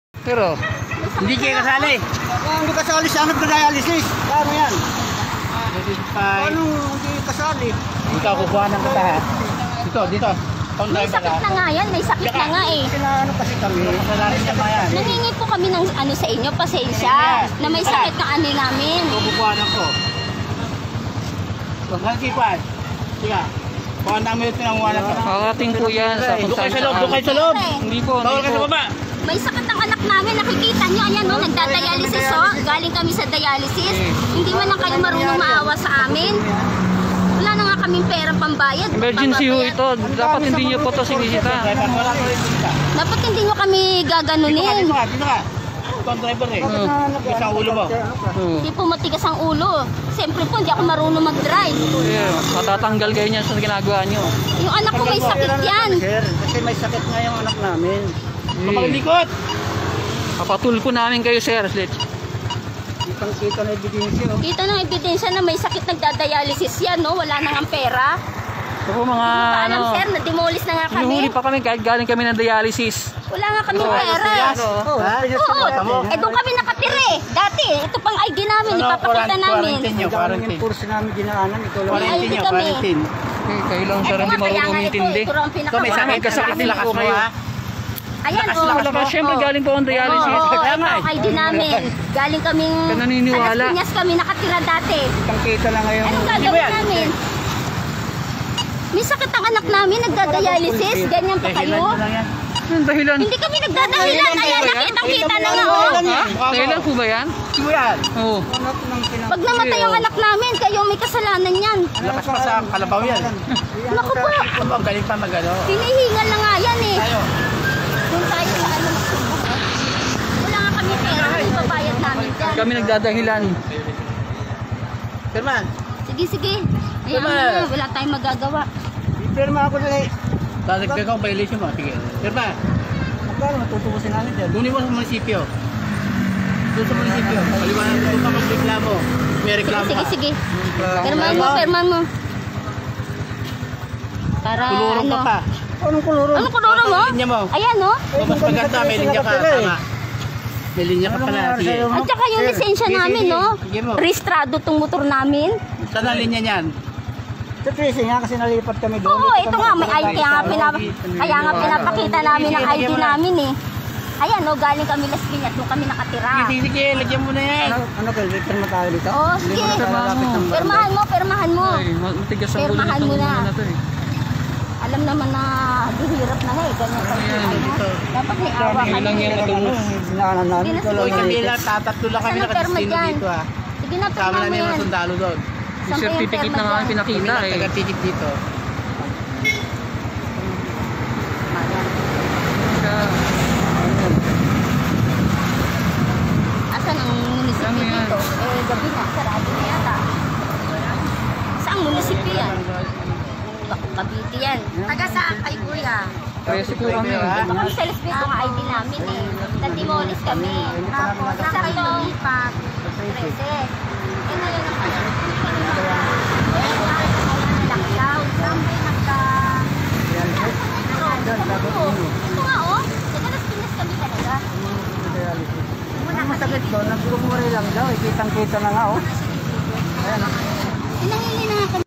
WALANG AWANG ENFORCER, TINIKITAN PA ANG DIALYSIS PATIENT
Si Tatang enforcer, Eduardo Mendoza, ng Bacoor Cavite, ay walang awang hinuli at tinikitan pa ang pasyenteng pauwi na mula sa katatapos lang na pagpapa-dialysis. Naghahabol ka ba ng quota Manong?

©️Michael Asidilla Makakita ng Kaunti